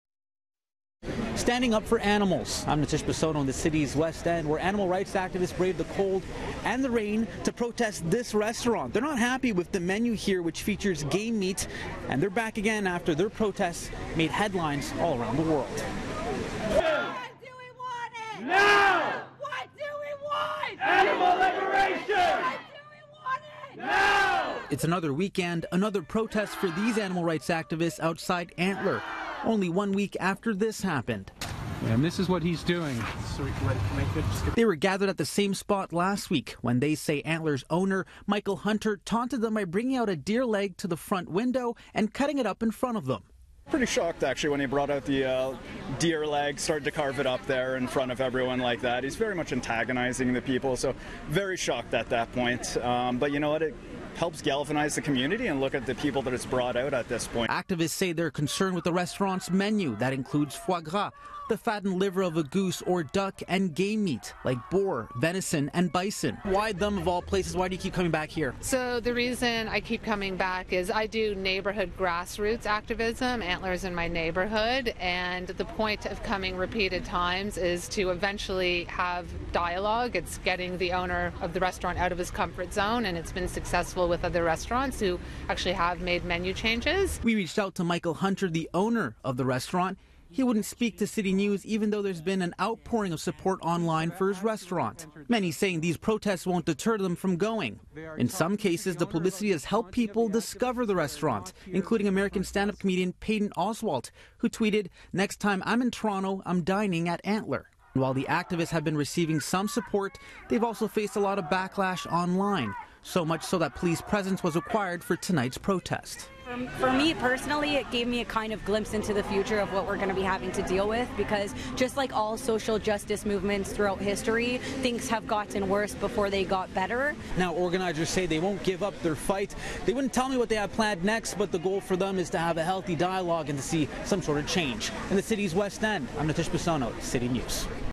Animal activists return to protest west end restaurant
Animal rights activists were back in front of Antler Kitchen & Bar Saturday night after their protest last week made international headlines. It appears the publicity may have both helped and hurt their cause.